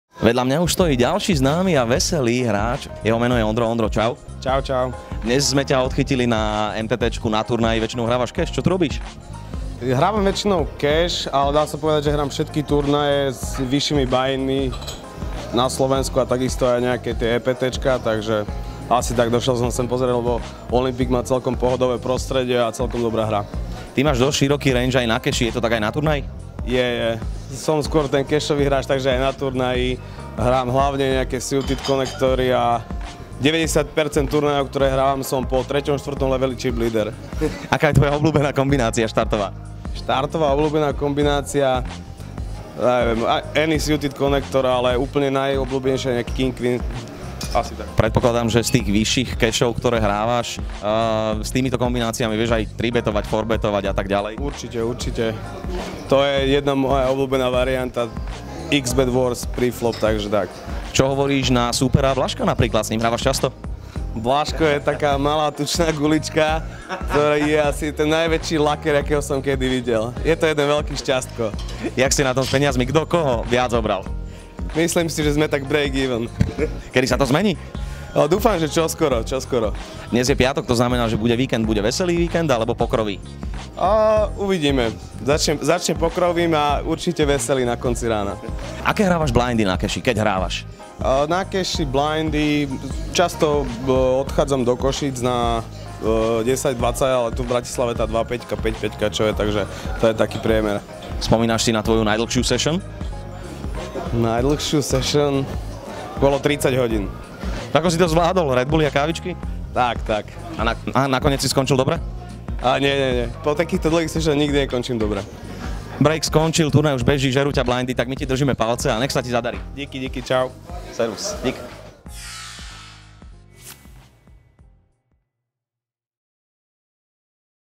Rozhovor - Ondro - olympic poker tour eurovea
PokerListTV sa  tentokrát podarilo dostať pred kameru známeho slovenského pokerového hráča Ondra,  ktorý v rozhovore hodnotí aj hru Mareka "žandára" Blažka. Viac sa dozviete vo videu.